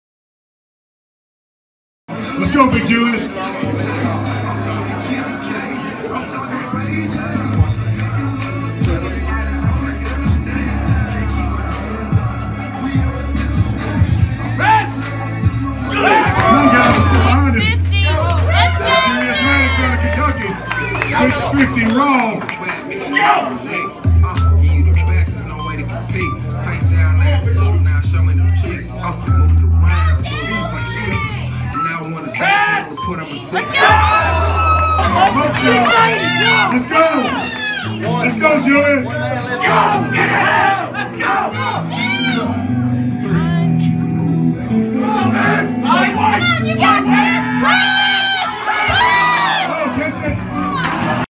Julius Maddox, 650 Raw Bench Press - All Three Attempts
Want to get stronger?